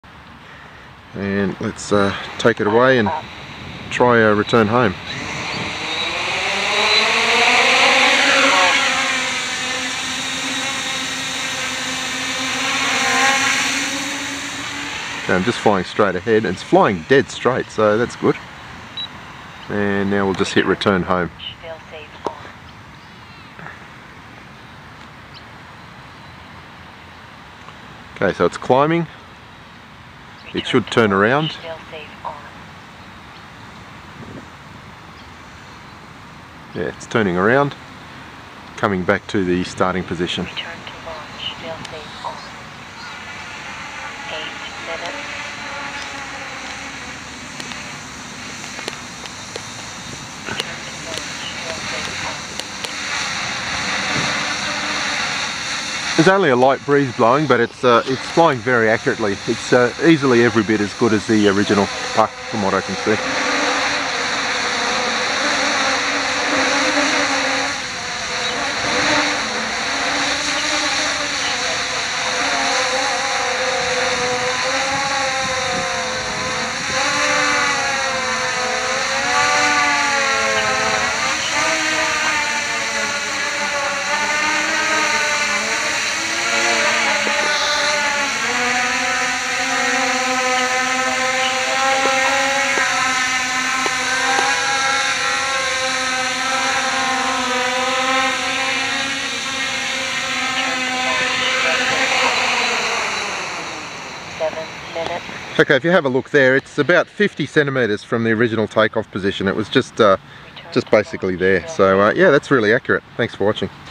Chinese GPS for NAZA RTH Test
This GPS works just like the original on my Naza Lite flight controller.